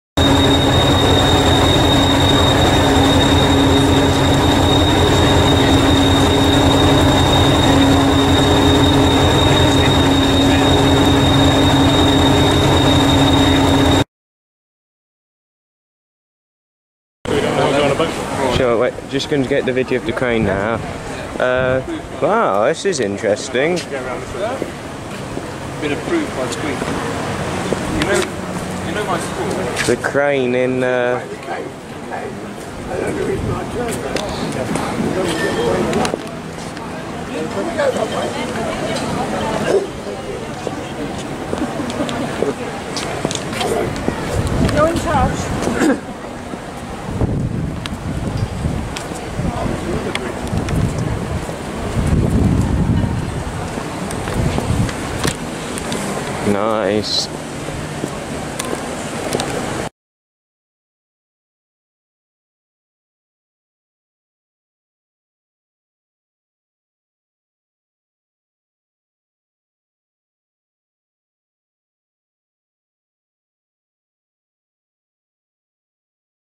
Foxy standing near the old fashioned crane in Gunwharf Quays, 22nd July 2018
Hello there everybody it's Foxy speaking and yes in this video there was me 'Foxy' standing alongside a really old fashioned looking stone-build crane in Gunwharf Quays in Portsmouth on a happy and sunny Sunday 22nd July 2018 in the afternoon. Nice to film things like that but incredibly rare to see these days :-/. But Gunwharf is a nice location. Industrial but nice :-). I don't know whether Gunwharf is industrializing with something but its generally crowded but not as crowded like London but there's always the interesting hussle and bussle to be seen there whenever I go there at times :-). Before I used to be really frightened of Gunwharf Quays and I was particularly fearful of Gunwharf Quays and Portsmouth when I first went there. This was back in August 2010 when I first went there but when I went there the second time in 2014, I got over my fear of going to Portsmouth and Gunwharf. So my fear and frightfulness is thankfully not sliding back into me whenever I think about or explore Portsmouth and Gunwharf :-).
From Foxy :-).